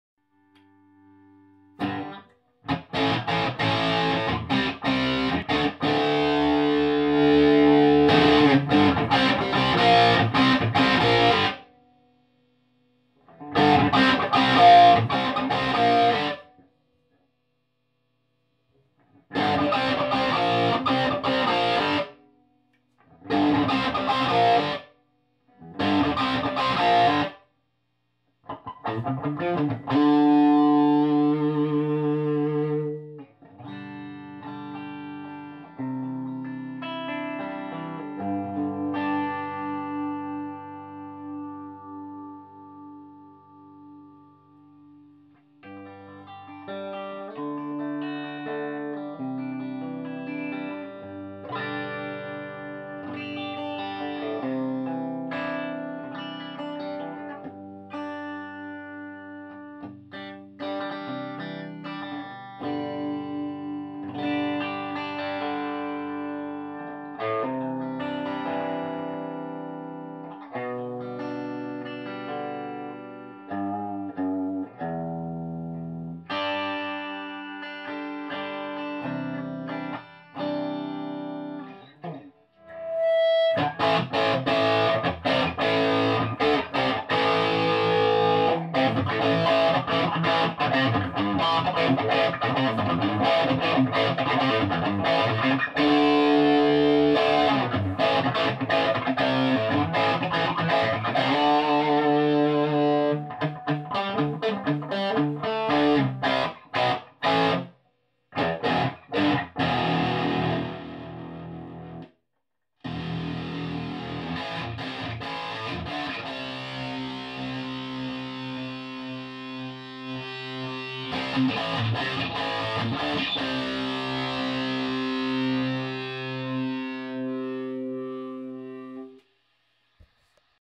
FJA modded Marshall JMP 2204 SS mod clip 3 low input
FJA Mods modified Marshall JMP 2204 with EL34's. SS mod with added depth mod. This clip thru the low input jack.